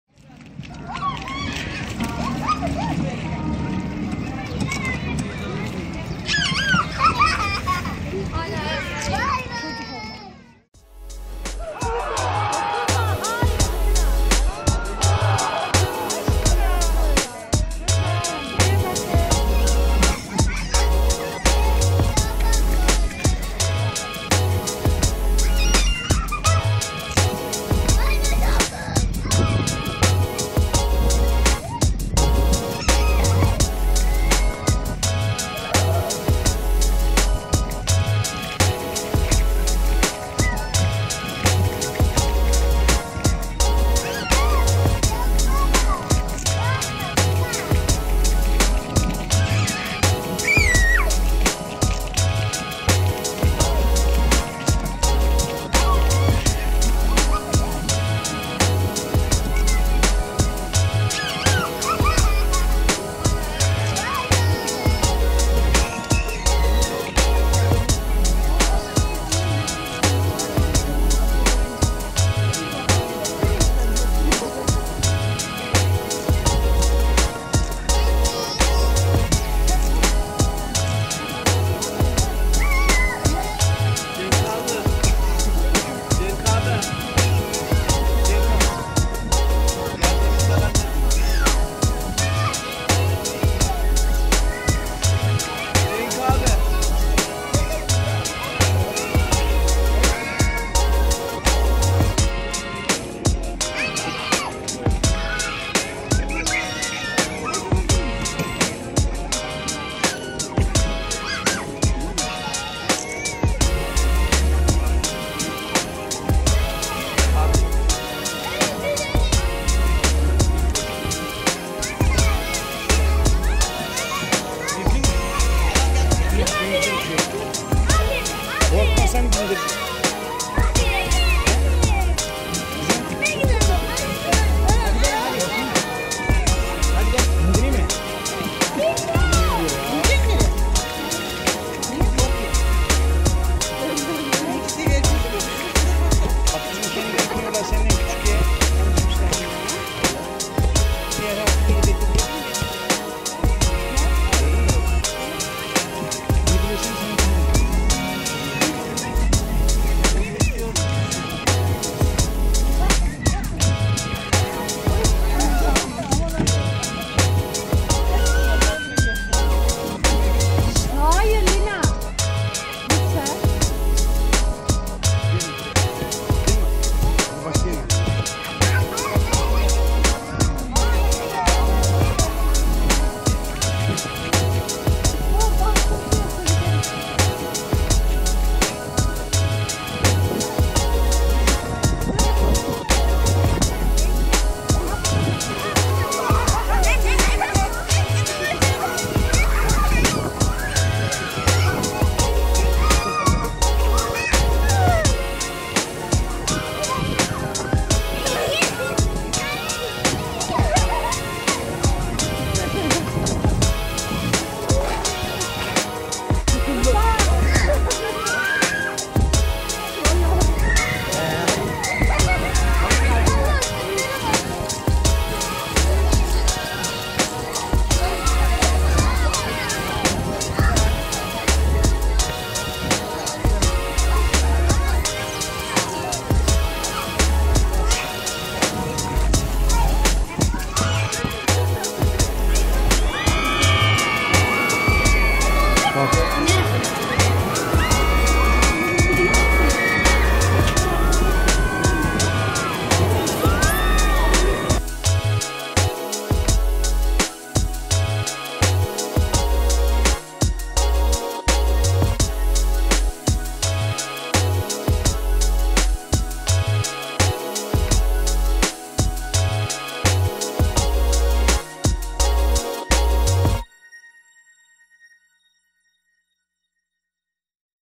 HAVUZDA ÇARPIŞAN BOT
Nas Lina Kuzeni ile Sudolu havuzda eğlendi.